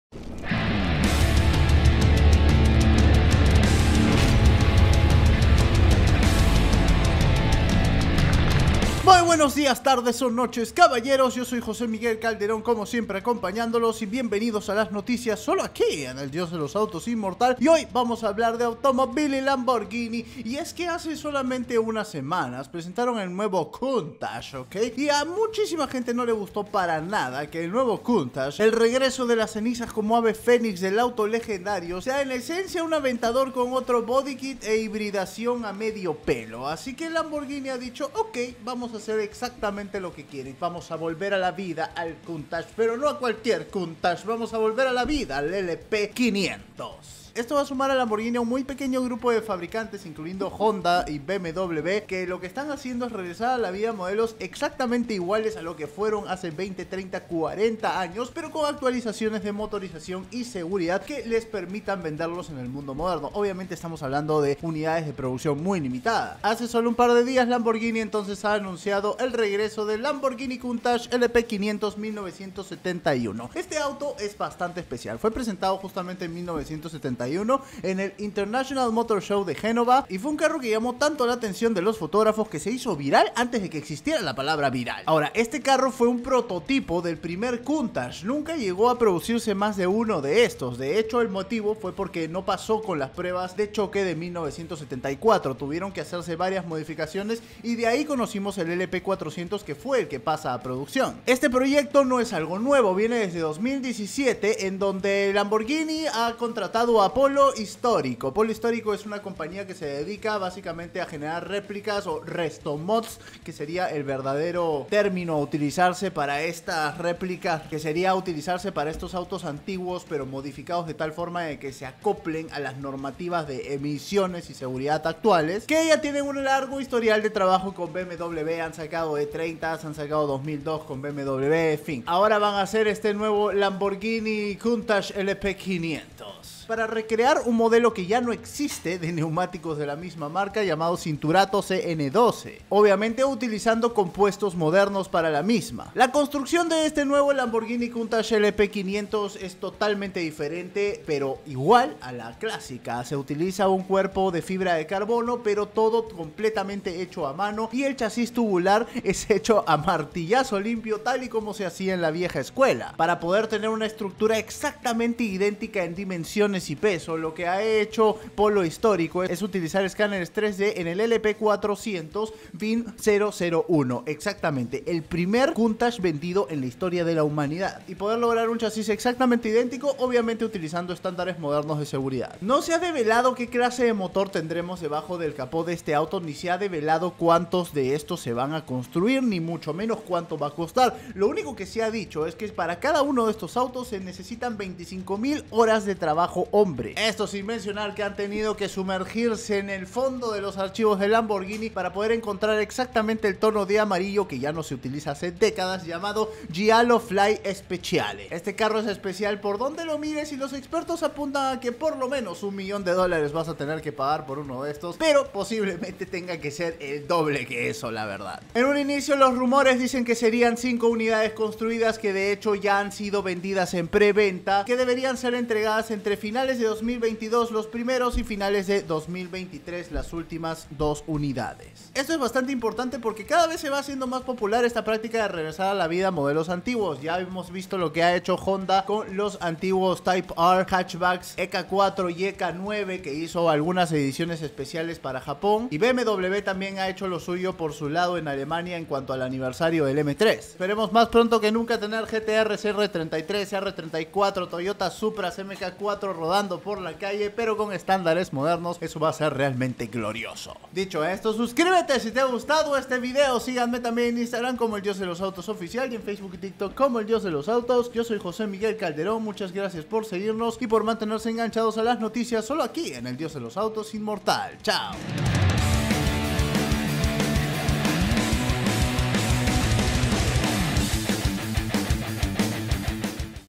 AHORA PUEDES COMPRAR 0KM AUTO DE 1971 - EL DIOS DE LOS AUTOS INMORTAL
LAMBORGHINI COUNTACH LP500 HA SIDO RECREADO EN UNA EXTREMADAMENTE LIMITADA EDICIÓN PARA COLECCIONISTAS CON ACTUALIZACIONES MODERNAS. NO ES BARATO, PERO SIN LUGAR A DUDAS ES MUCHÍSIMO MÁS ECONÓMICO QUE CONSEGUIR UNO DE ESTOS AUTOS QUE ES LITERALEMTNE UN UNICORNIO DE SUPERAUTO ITALIANO.